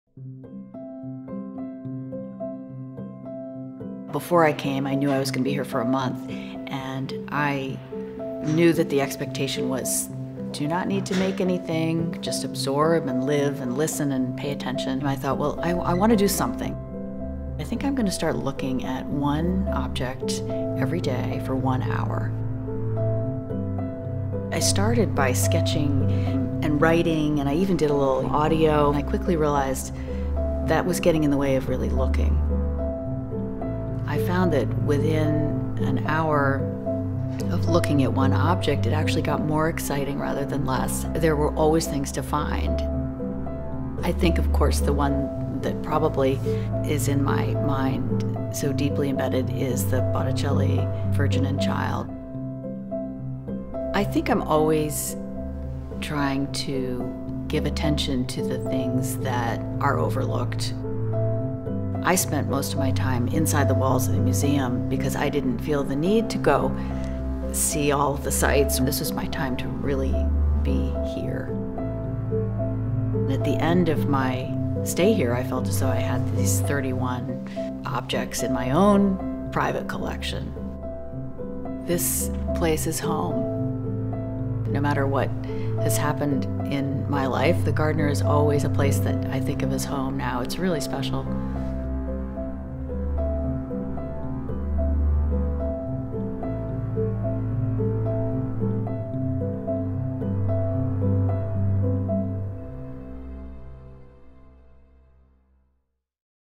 Artist-in-Residence: Rachel Perry
While in residence at the Gardner, she decided to spend an hour each
day gazing at a different object. She was struck by the overwhelming
number of religious paintings and sculptures. In response to her close scrutiny and counting of the halos at the Gardner, Perry produced 445 unique drawings that suggest constellations.

Perry's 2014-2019 piece, Halos, was featured as part of the exhibition, In the Company of Artists: 25 Years of Artists-in-Residence, on view at the Isabella Stewart Gardner Museum October 17, 2019, through January 20, 2020